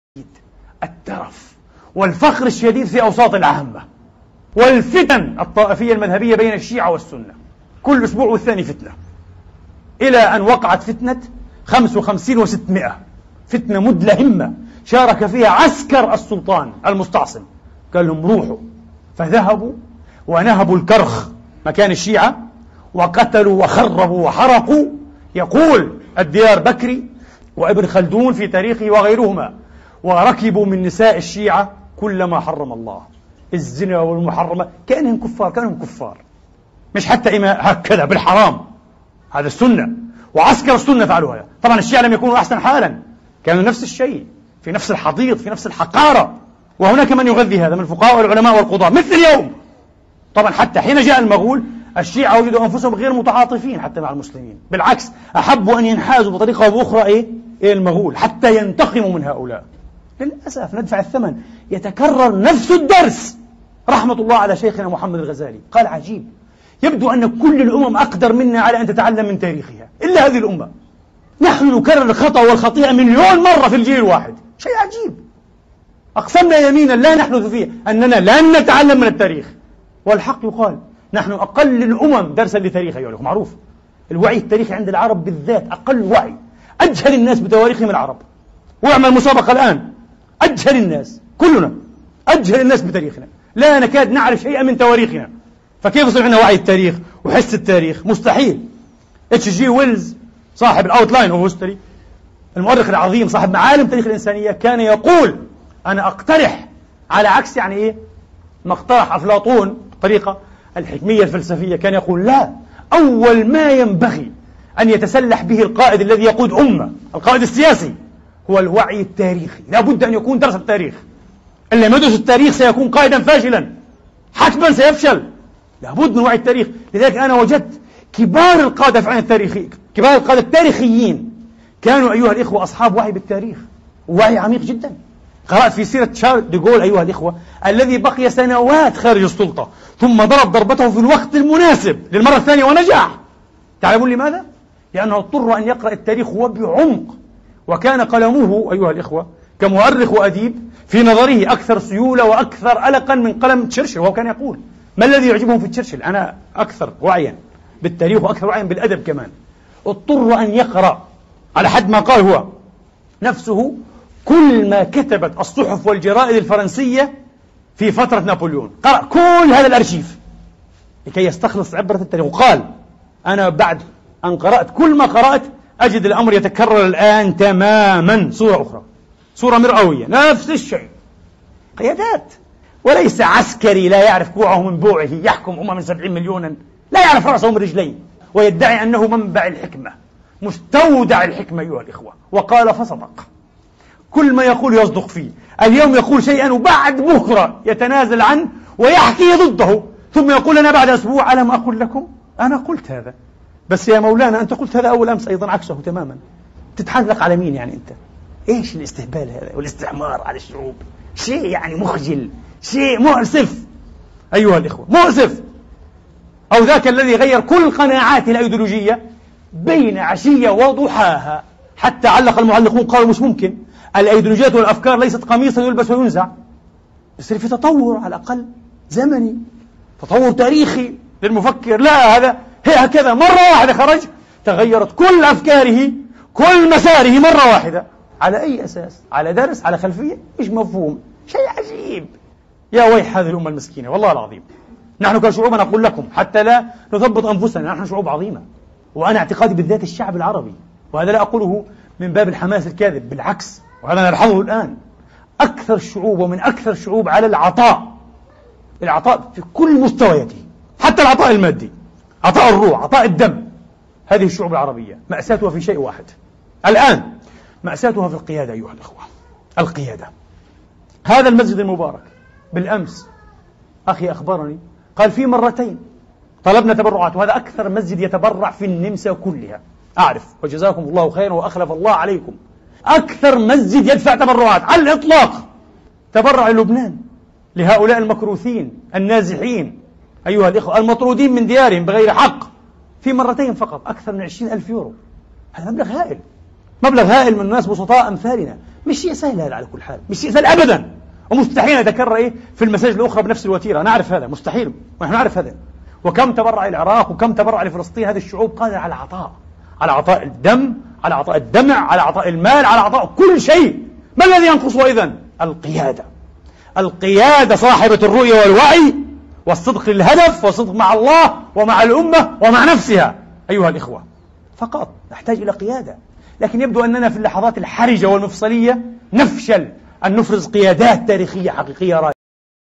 نحتاج الى قادة لديهم وعي تاريخي..!!! l د.عدنان ابراهيم
من خطبة أمة لاتبيد ... قصتنا مع المغول:
شكر الى الاخ ابو جبريل